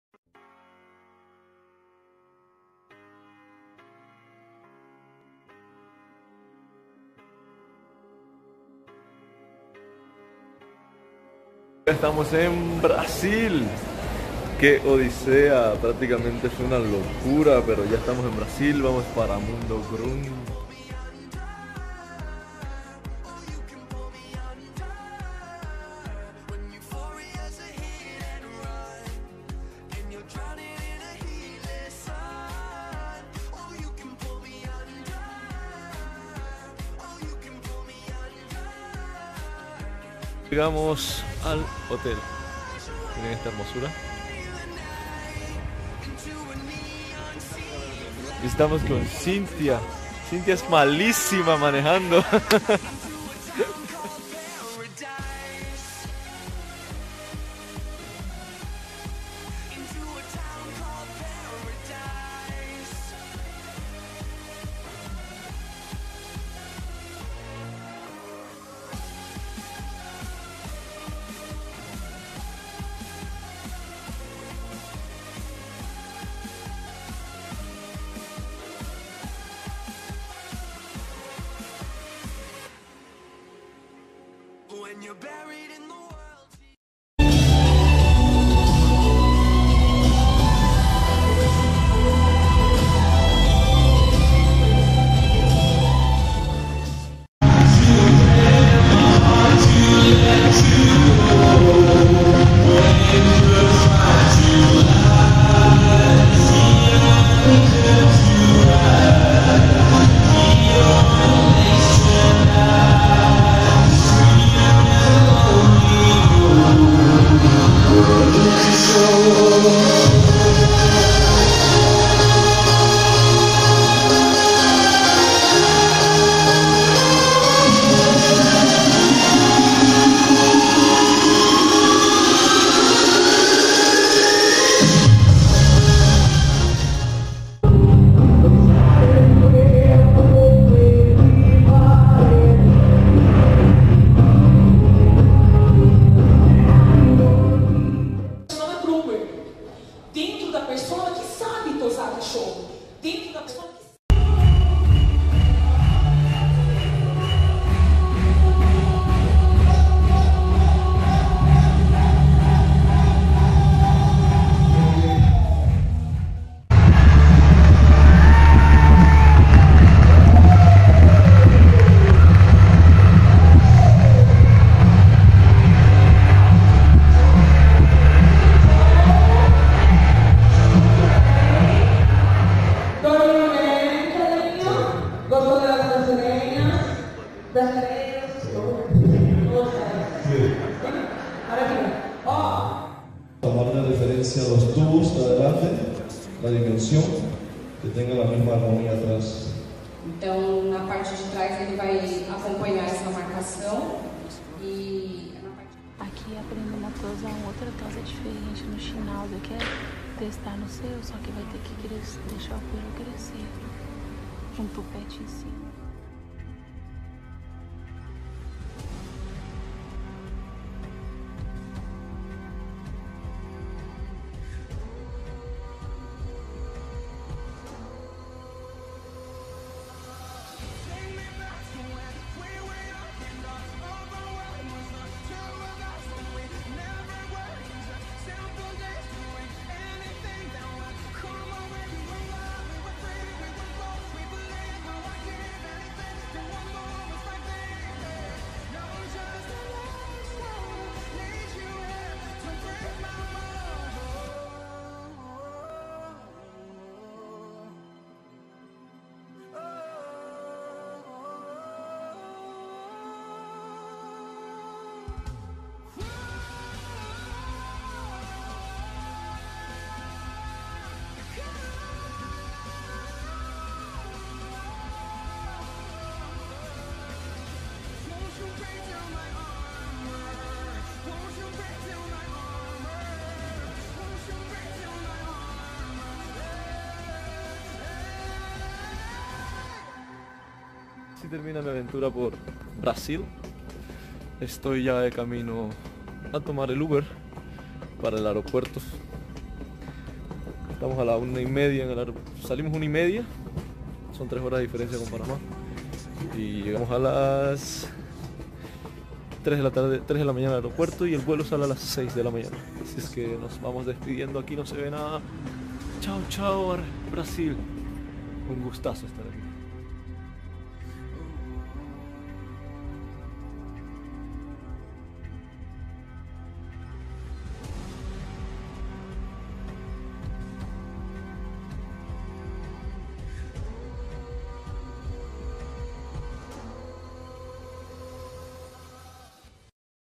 Mundo Groom 2019 / Brasil
Un poco de mi experiencia y participación en el Evento Mundo Groom, realizado en Sao Paulo Brasil Septiembre 2019